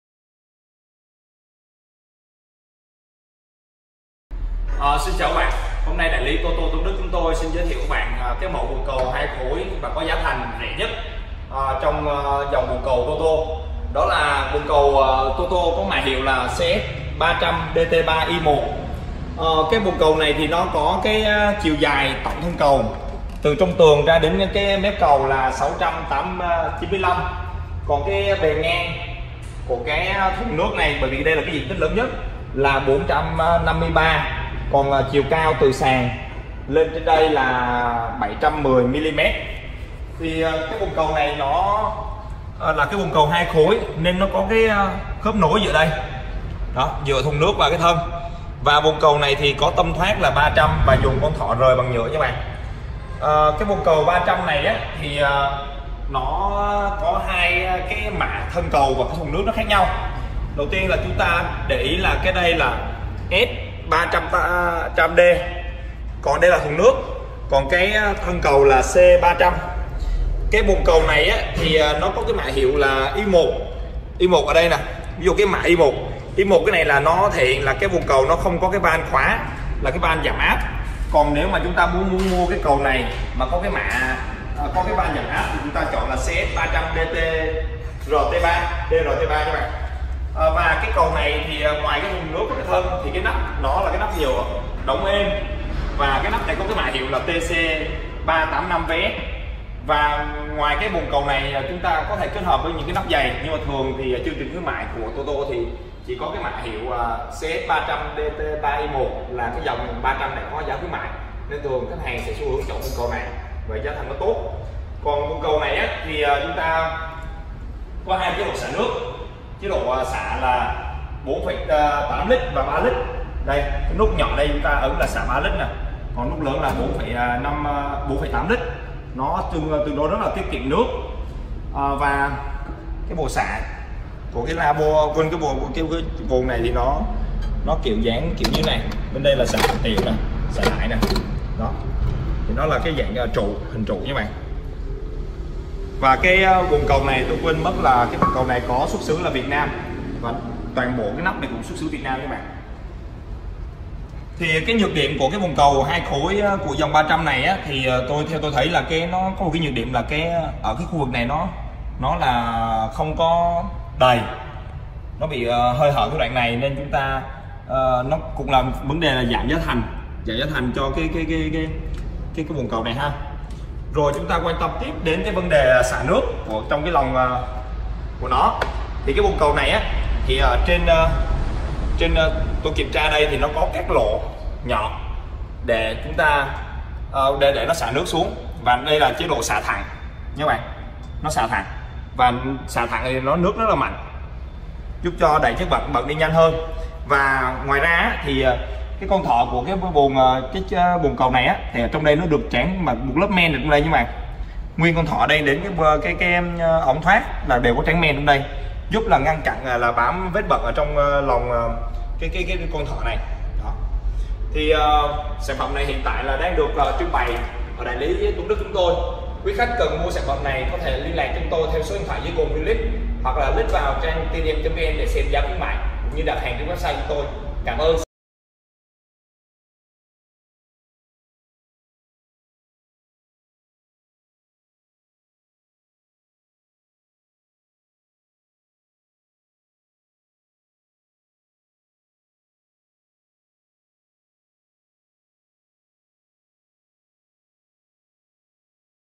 TDM.VN | Review bồn cầu 2 khối TOTO CS300DT3Y1 (C300Y1 + S300D) bàn cầu giá rẻ nhất thị trường
Thông tin cơ bản bồn cầu TOTO CS300DT3Y1
Bồn cầu của thương hiệu thiết bị vệ sinh TOTO
Tên sản phẩm: Bồn cầu TOTO CS300DT3Y1
Bồn cầu TOTO 2 khối nắp đóng êm TC385VS
Hệ thống xả: Vành Rim, Siphon
Xả 2 chế độ
Lượng nước xả: 4.8/3L
Thiết kế: Thân dài
Tâm xả: 305mm
Kích thước: 693x451x763 mm
Áp lực nước: 0.05MPa~0.75MPa
Màu sắc: Màu trắng
Xuất xứ: Việt Nam
Mã sản phẩm cũ: CST300DS
Tính năng bồn cầu TOTO S300D
Thiết kế sang trọng, hiện đại
Nắp đóng êm

Xuất xứ Việt Nam - Bồn cầu bao gồm 3 bộ phận
Thân cầu: C300Y1
Két nước: S300D
Nắp bồn cầu: TC385VS
Bản vẽ kỹ thuật xí bệt TOTO C300